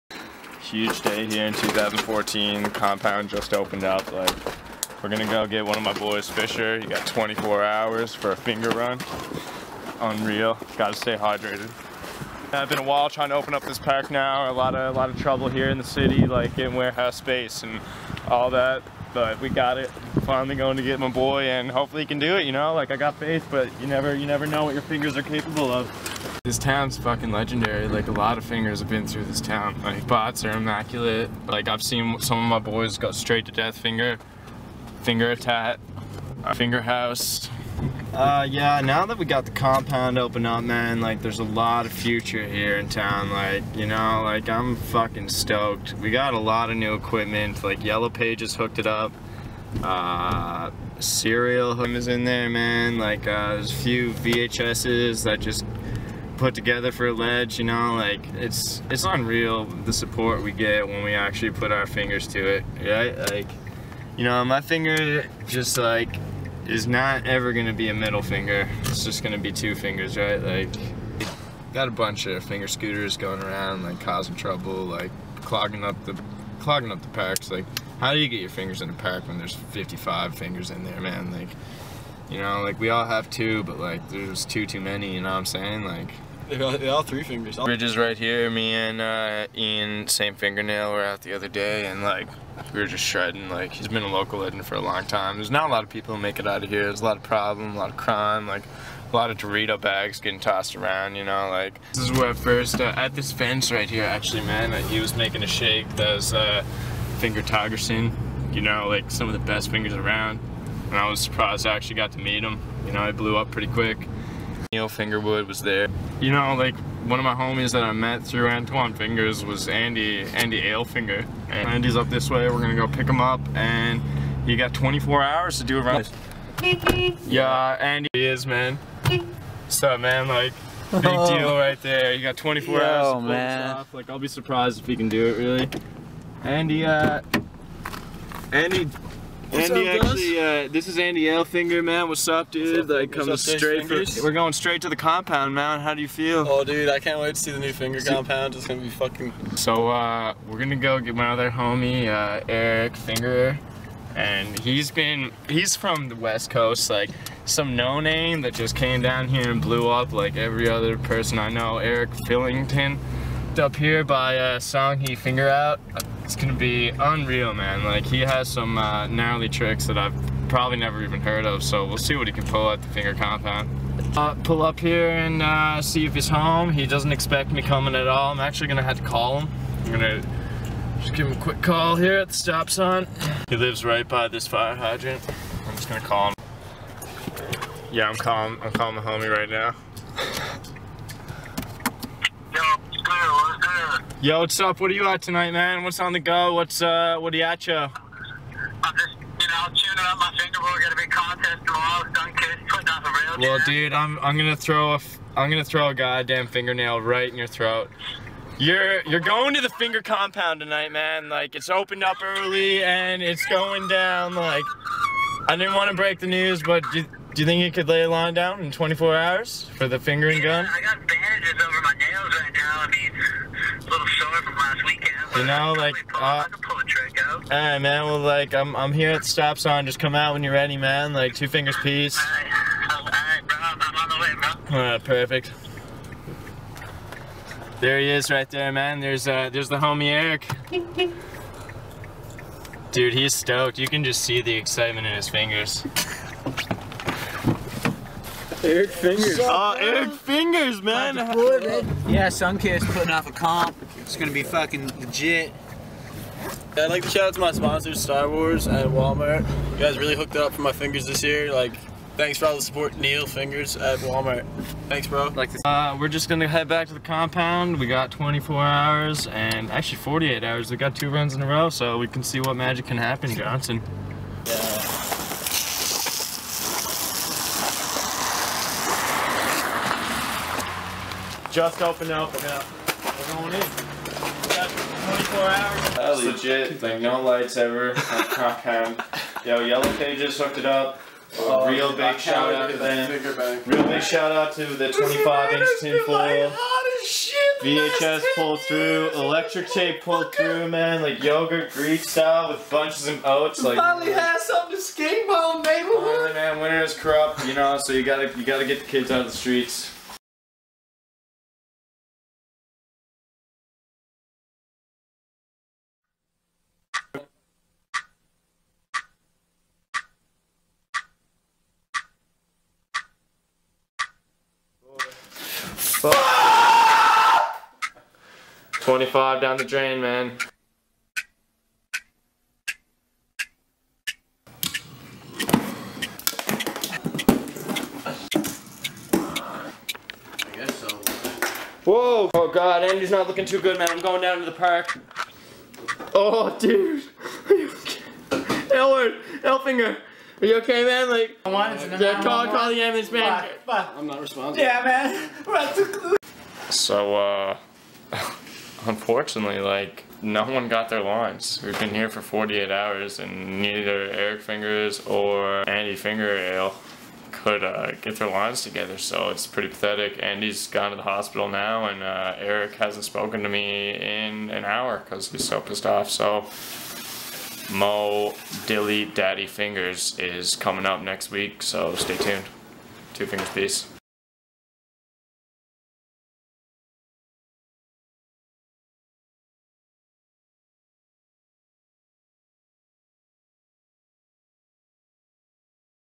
Cardboard Compound the Documentary ( FINGER AND GUN 2014)
Steve Finger takes us on an all exclusive tour of the finger compound neighbour hood and picks up finger legends Andy ale Fnger and Erik Finger.